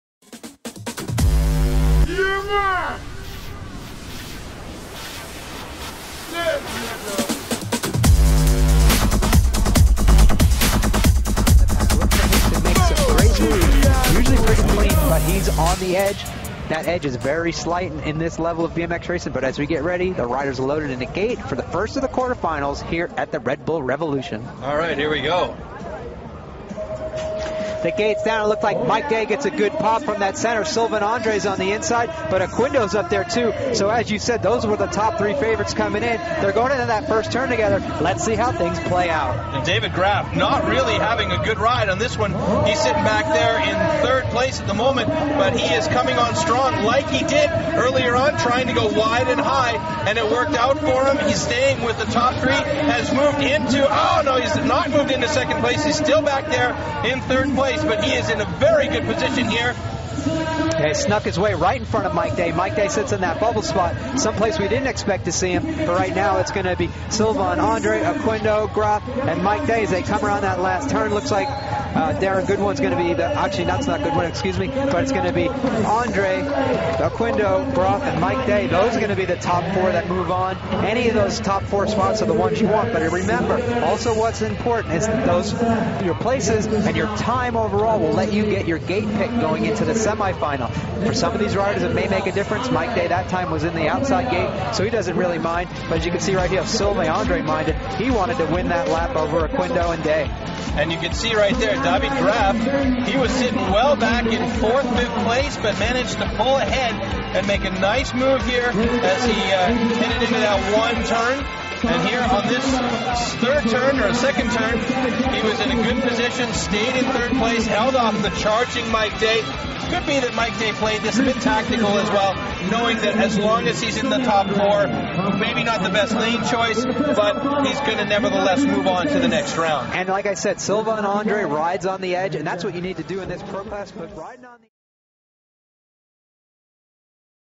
Red Bull R.Evolution Berlin 2012 - Quarter Finals Heat 1
Redbull R.Evolution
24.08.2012
Mellowpark Berlin

[566] Carlos Mario OQUENDO ZABALA (COLUMBIA)
[39] Sylvain ANDRE (FRANCE)
[92] Martjin JASPERS (NEDERLAND)
[365] Mike DAY (USA)
[48] David GRAF (SWITZERLAND)
[327] Jordy VAN DER HEIJDEN (NEDERLAND)
[161] Mike GONZALEZ
[170] Luca VIRDIS